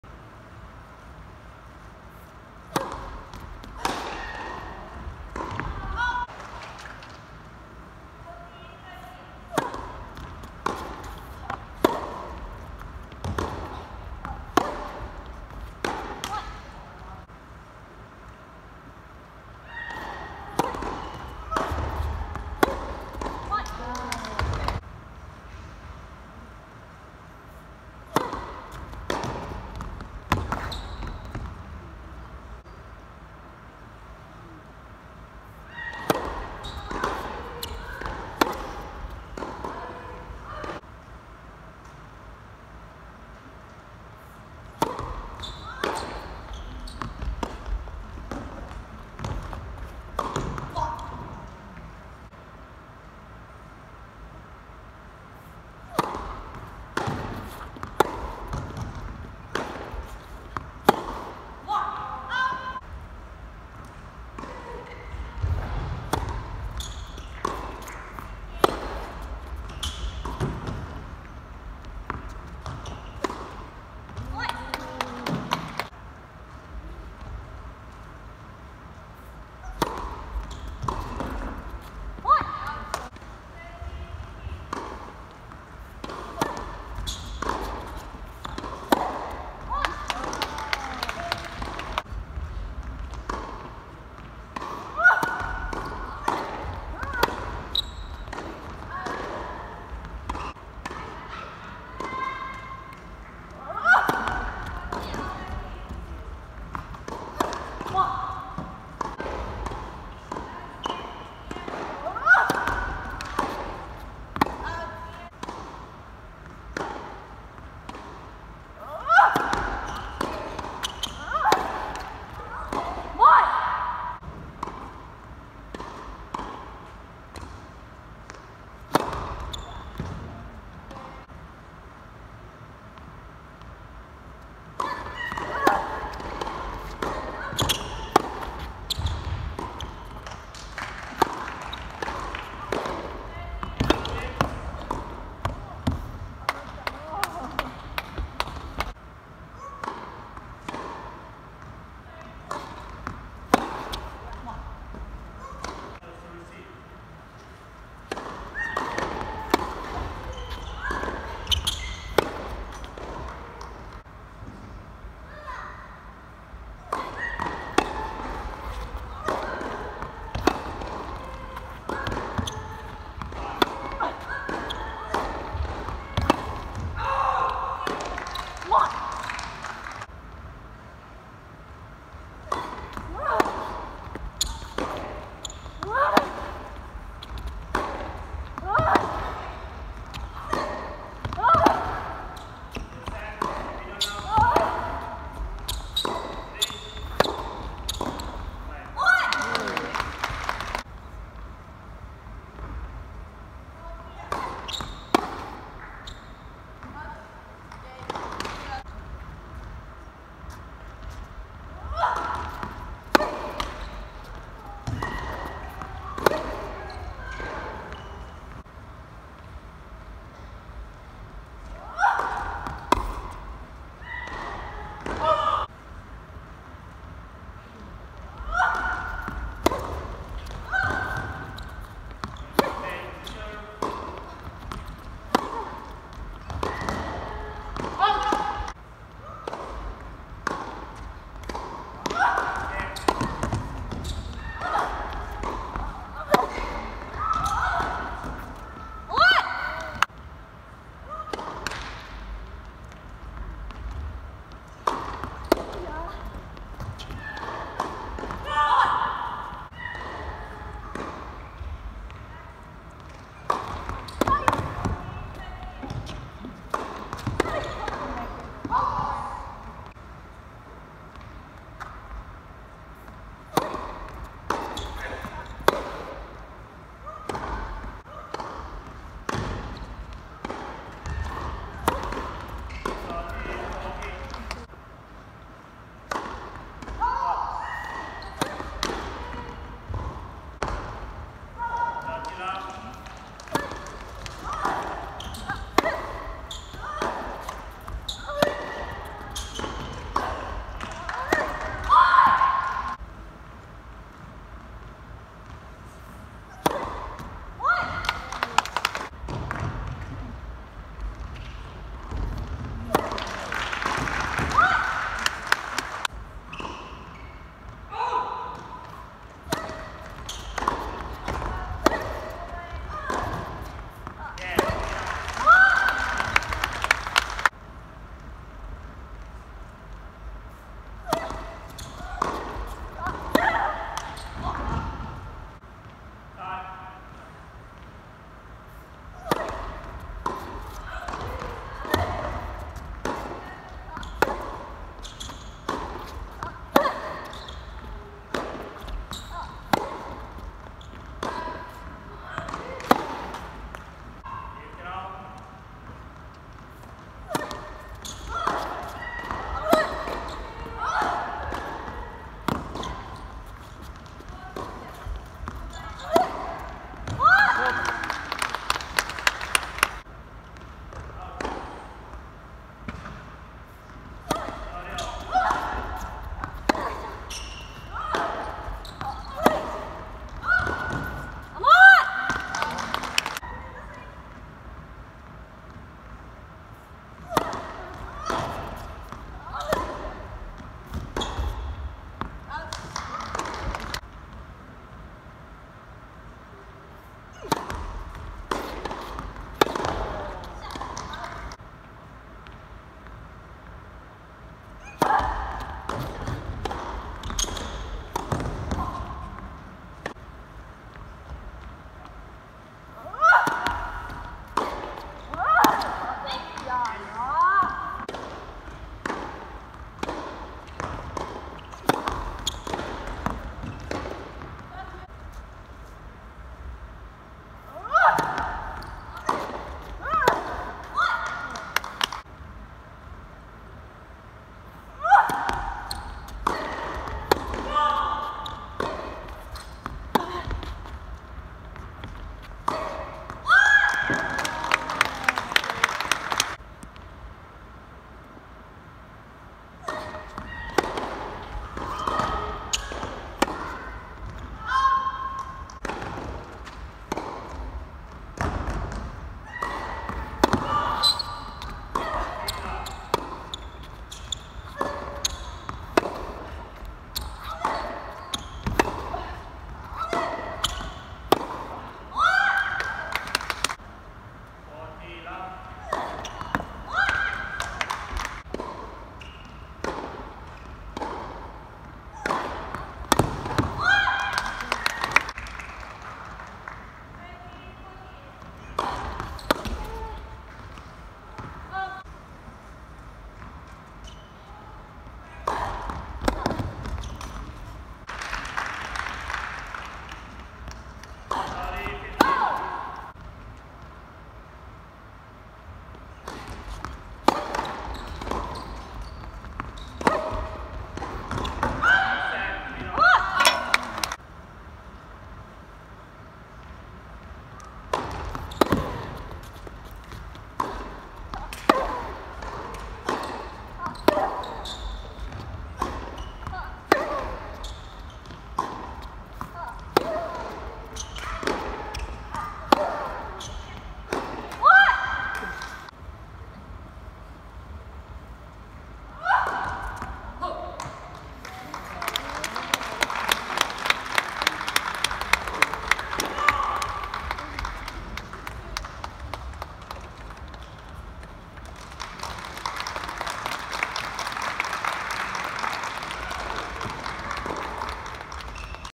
③岡村恭香 vs Fangzhou Liu ITF W60 2019 第3セット
「岡村恭香 vs Fanzhou Liu 第1&2セット」⬇︎

🎾コメント欄にナイスショットリンク集🎾

World Tennis Tour W60 2R

（Q）岡村恭香（JPN🇯🇵）422位
（3）Fangzhou Liu（CHN🇨🇳）178位
※ランキングは当時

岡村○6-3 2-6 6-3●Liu

※ファーストサーブのフォルトはカットしています。
※セカンドサーブから始まるポイントはスコアの右横に「2nd」と記載しています。